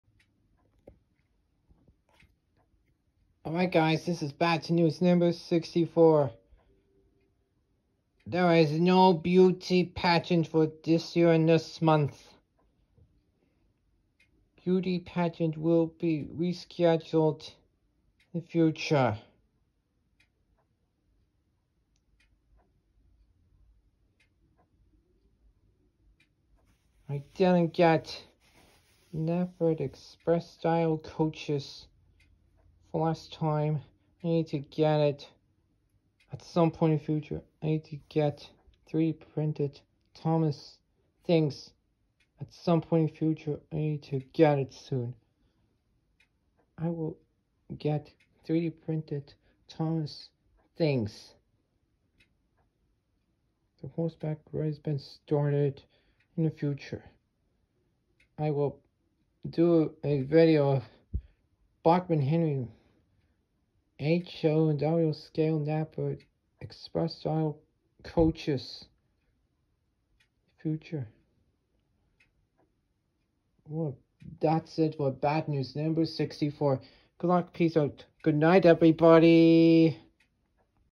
Bad News #64
There is no Beauty Pageant for this year and this month! Beauty Pageant needs to be rescheduled in the future! I will get 3D Printed Thomas Things at some point in the future! I will do a Video of Bachmann Henry with Knapford Express Style Coaches at some point in the future!